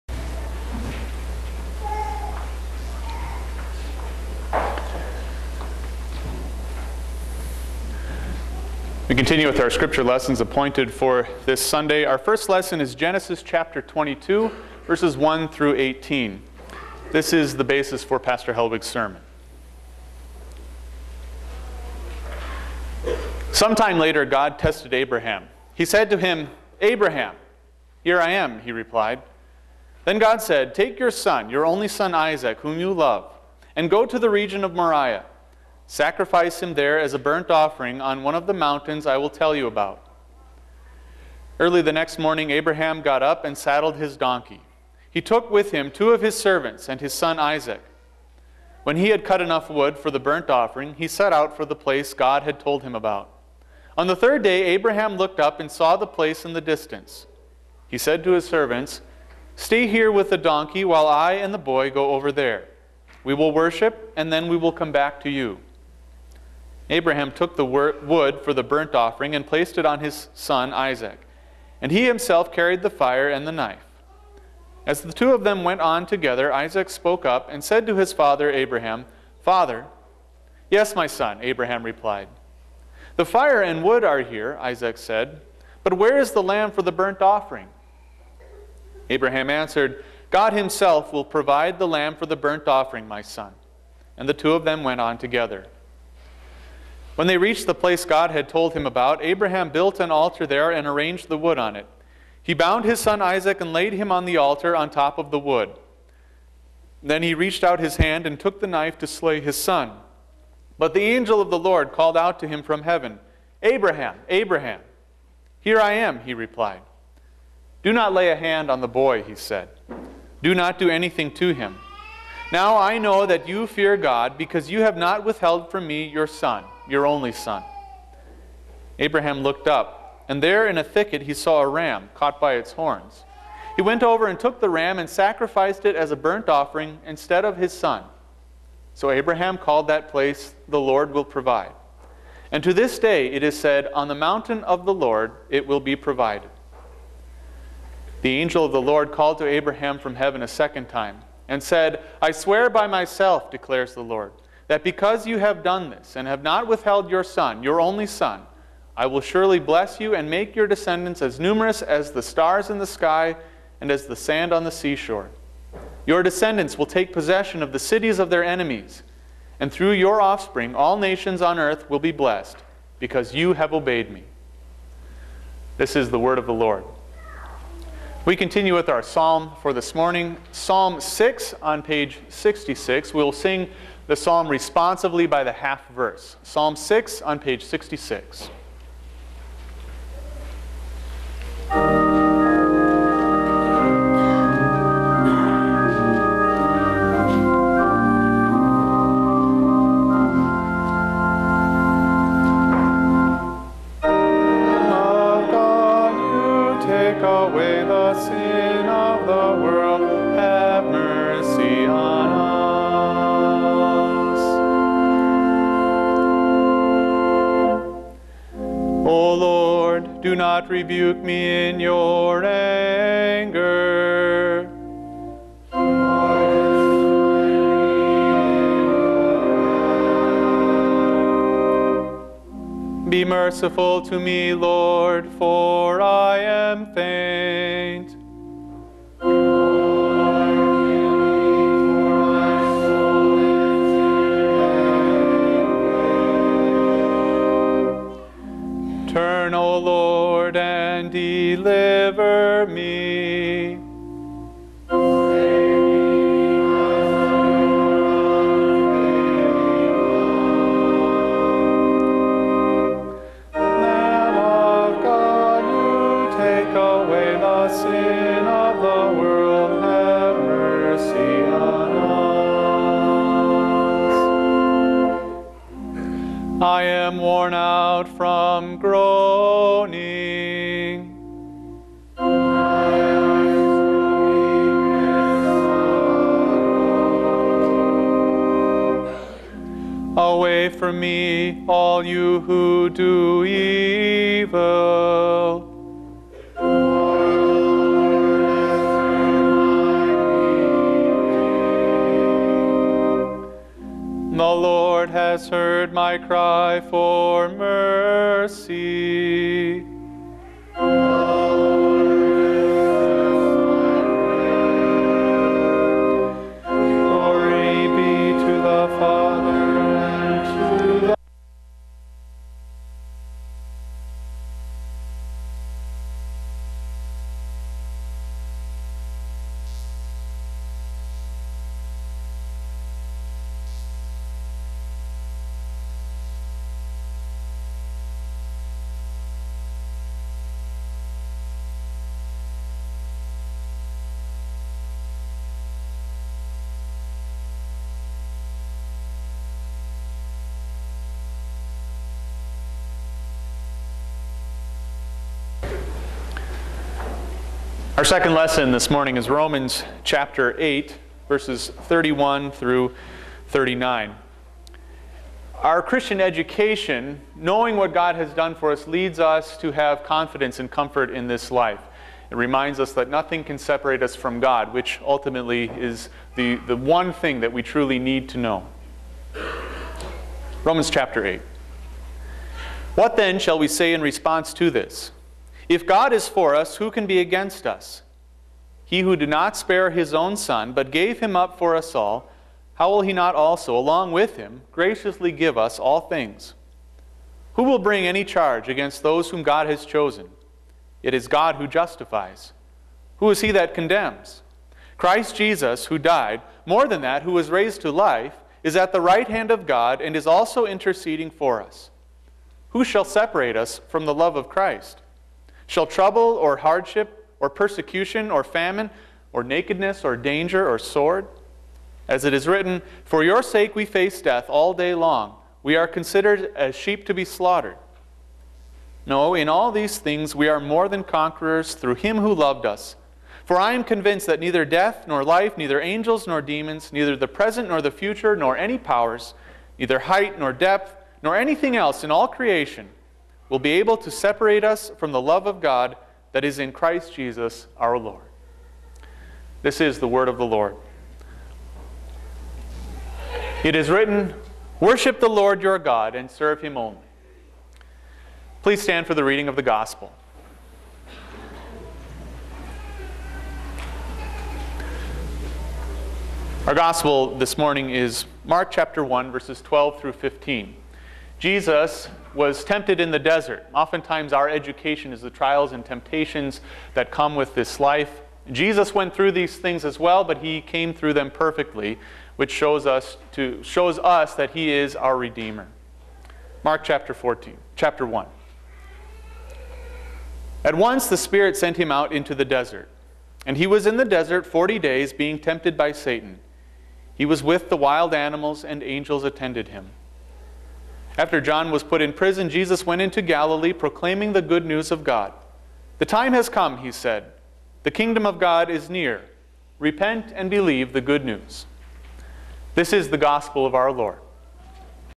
02/22/15 1st Lesson (sermon text), Psalm, 2nd Lesson, and Gospel (Pastor Koester)
1st Lesson - Genesis 22:1-18 (sermon text)
Psalm 6
2nd Lesson - Romans 8:31-39